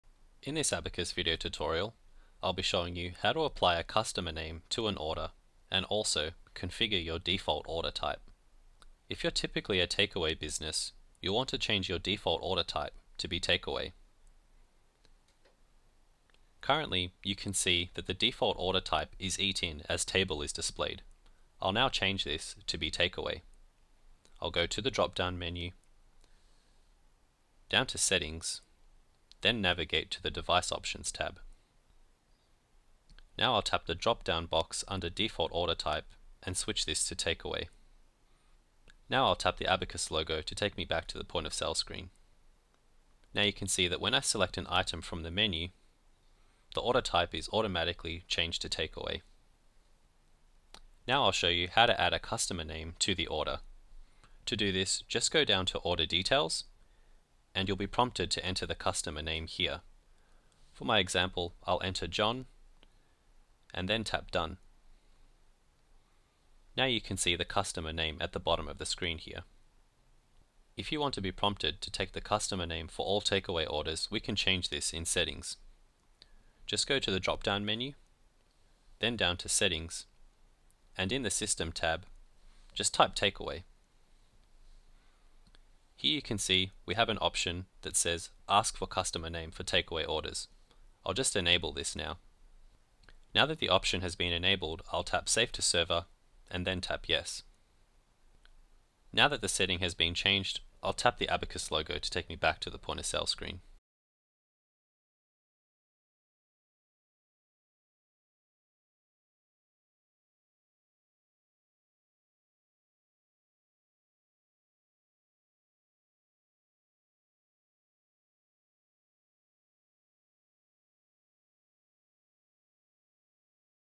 9. Setting the default order type and adding a customer name to an order
In the ninth video of our frontend training series, we'll cover the following:

- Setting the default order type to eat in or takeaway
- Starting a takeaway order with a customer name
- Setting takeaway orders to always prompt for the customer name